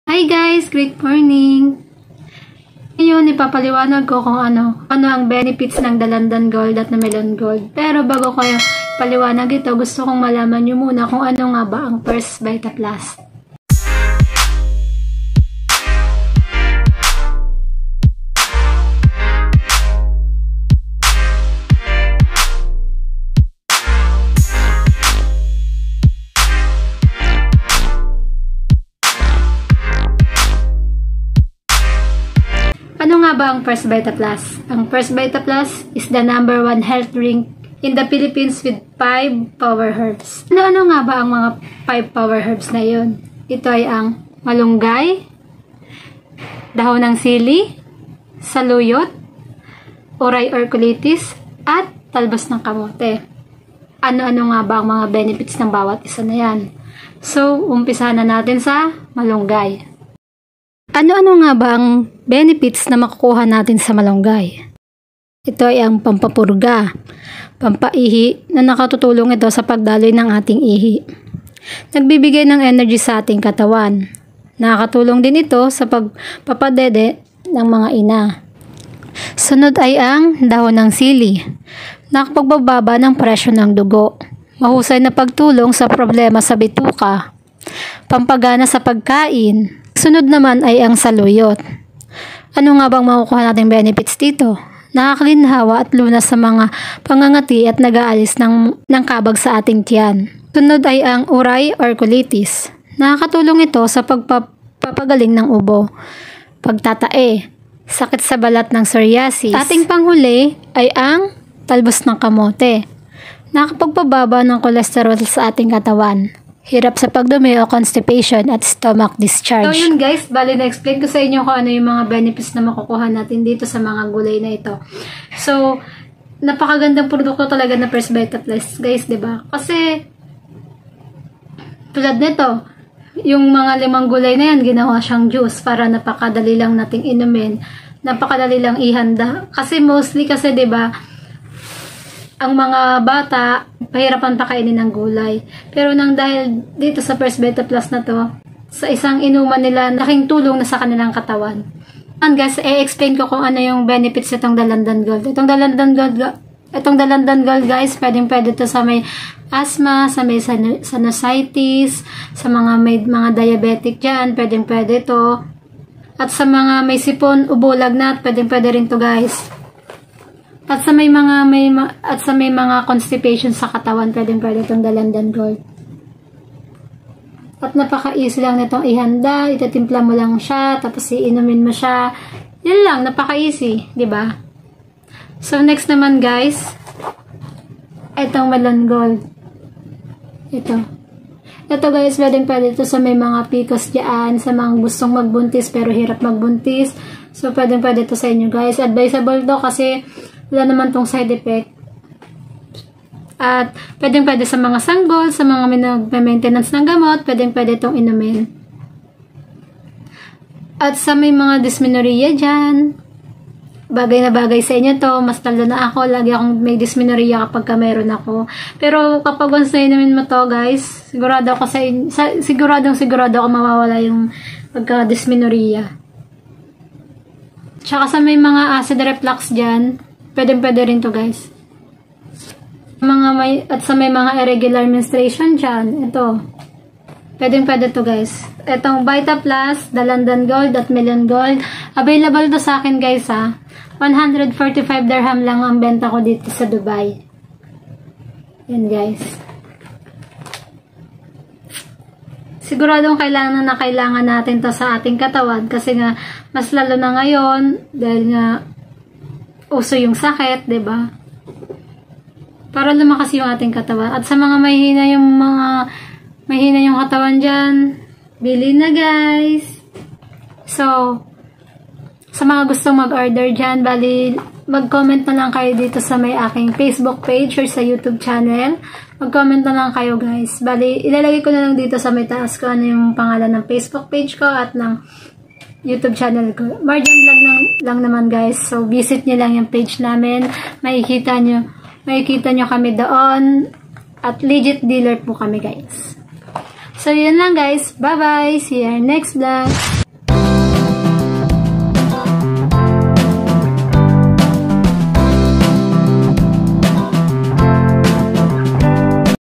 ANO ANG MGA BENEFITS NG FIRST VITA PLUS DALANDAN GOLD AT MELON GOLD
napaka gandang produkto ng first vita plus dahil makakatulong talaga para palakasin ang ating immune system